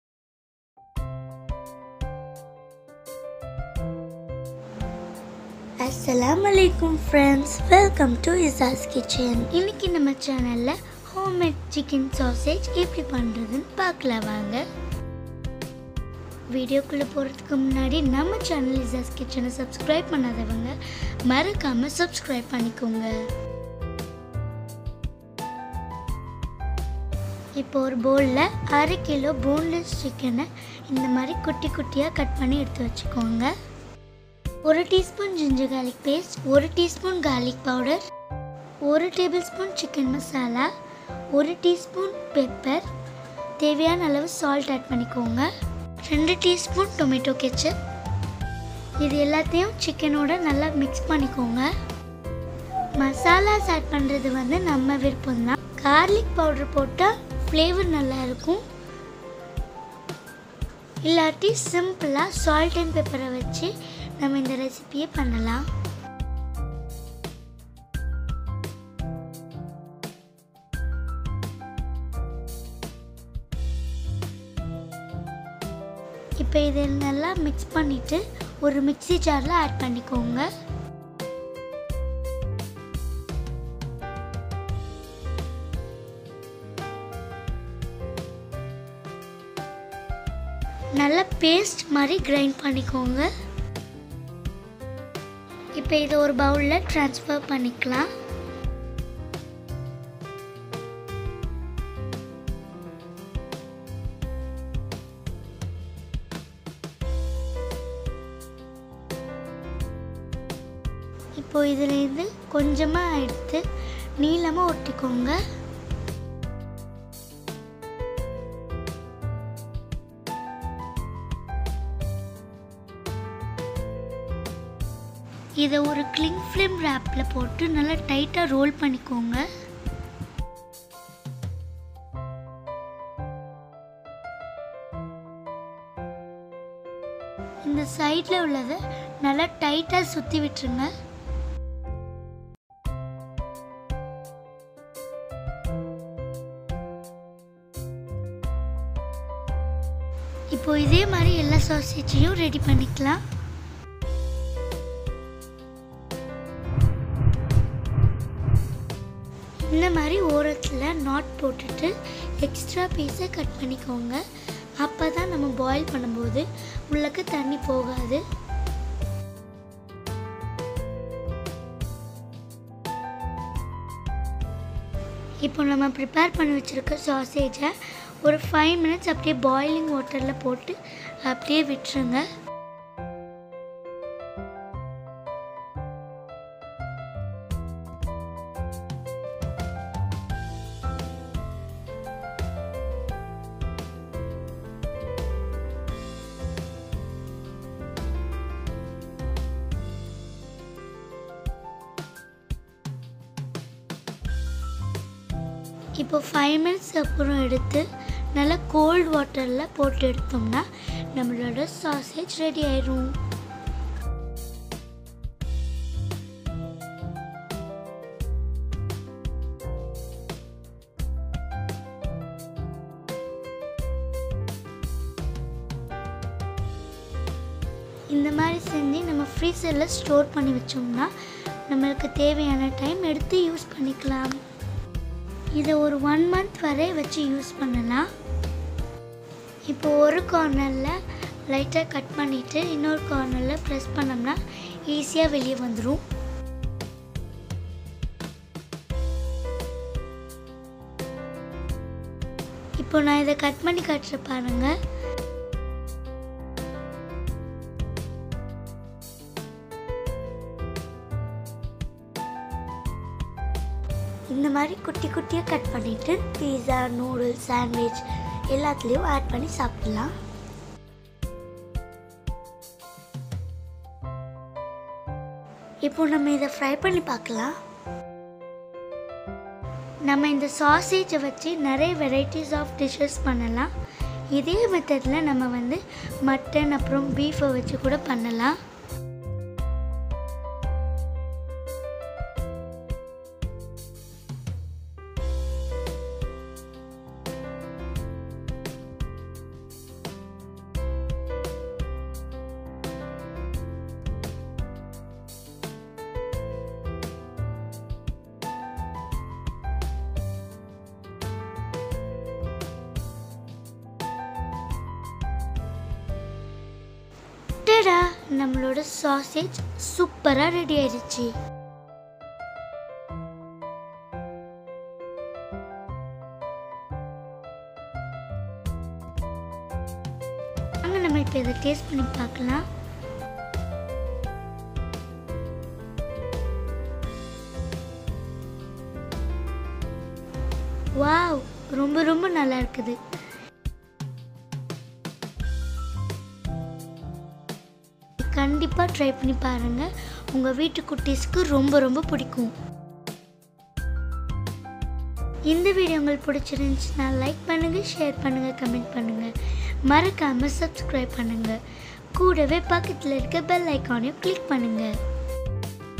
Homemade Chicken Sausage in Tamil | Homemade chicken hot dogs
In this video you can see how to make chicken sausage easily at home in Tamil.
It is an easy way to make sausages at home without any preservatives and that are tastier than the sausages available in supermarket.